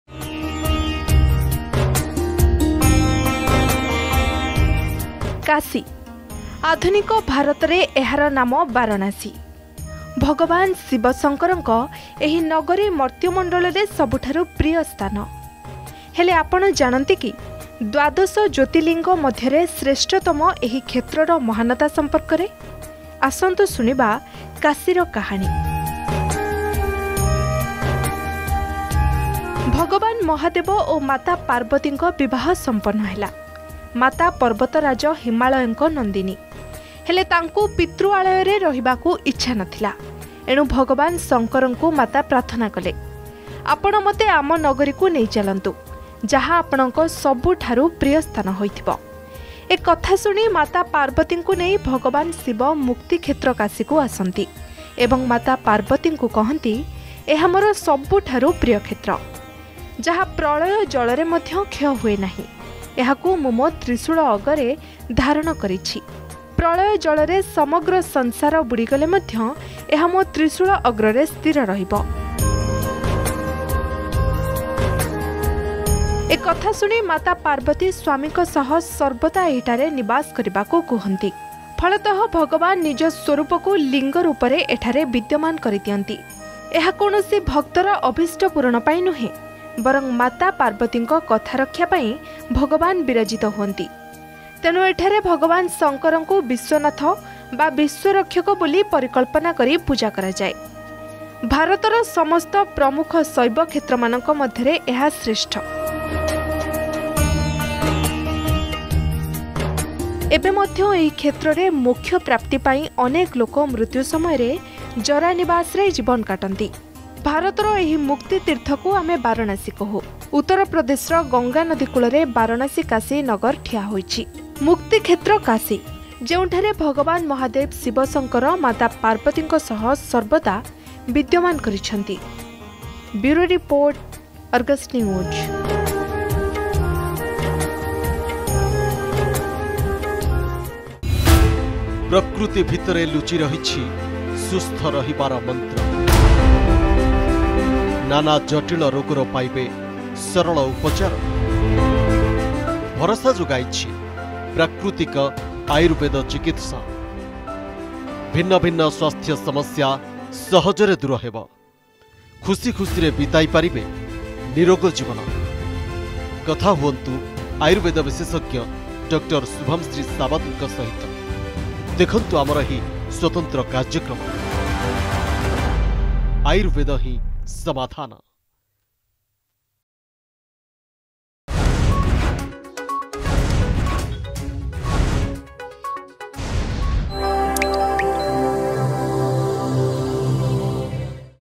କାଶୀ.. ଆଧୁନିକ ଭାରତରେ ଏହାର ନାମ ବାରଣାସୀ
କାଶୀ । ଆଧୁନିକ ଭାରତରେ ଏହାର ନାମ ବାରଣାସୀ। ଭଗବାନ ଶିବ ଶଙ୍କରଙ୍କ ଏହି ନଗରୀ ମର୍ତ୍ତମଣ୍ଡଳରେ ସବୁଠାରୁ ପ୍ରୀୟ ସ୍ଥାନ। ହେଲେ ଆପଣ ଜାଣନ୍ତି କି ଦ୍ବାଦଶ ଜ୍ୟେତିଲିଙ୍ଗ ମଧ୍ୟରେ ଶ୍ରେଷ୍ଠତମ ଏହି କ୍ଷେତ୍ରର ମହାନତା ସମ୍ପର୍କରେ।